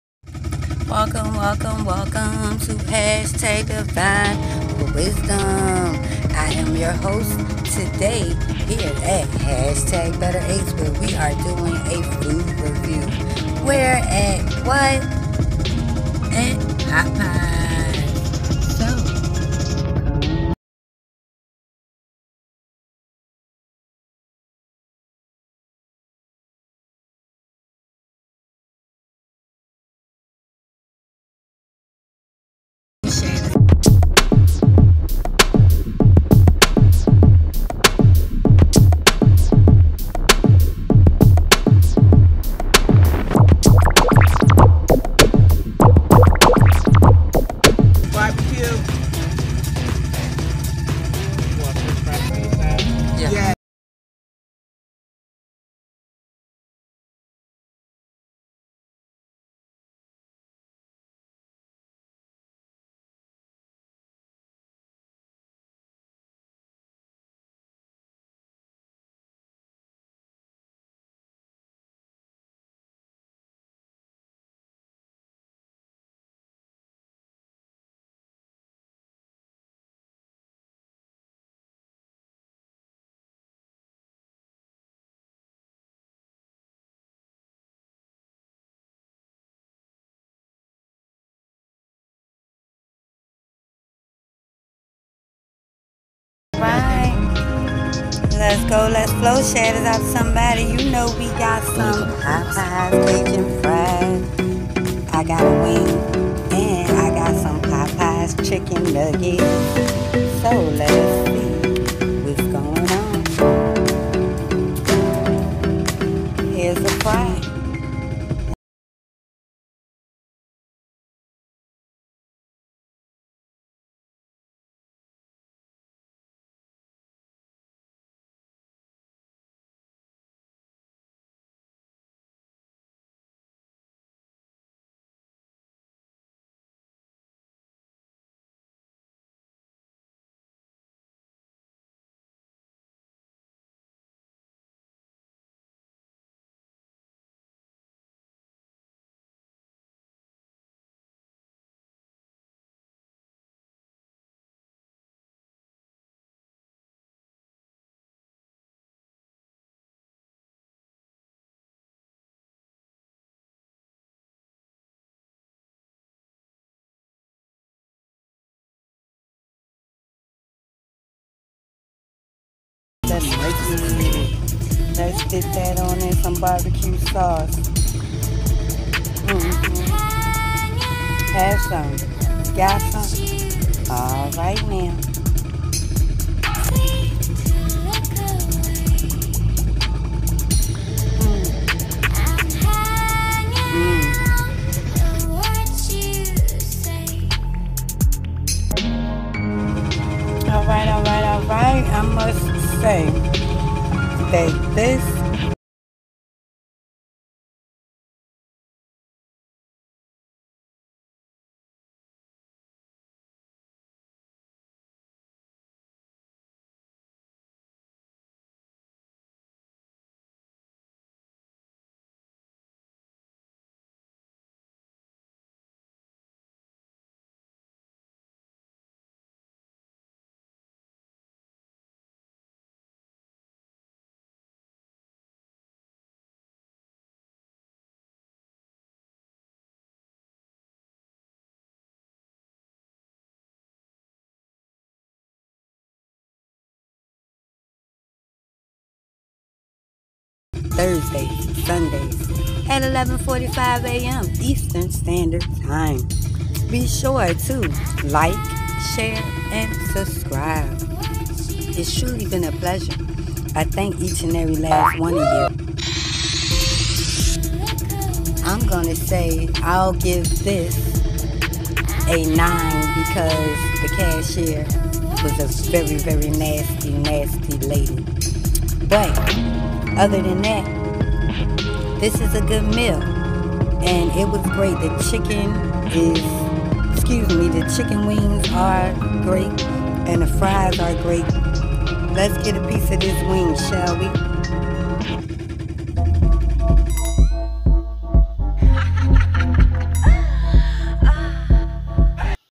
PopeyeFoodReview
Join me in this epic food review where

@DvineWizdom is trying the new NUGGETS!

From Popeyes enjoy I send you peace &

blessings to be upon.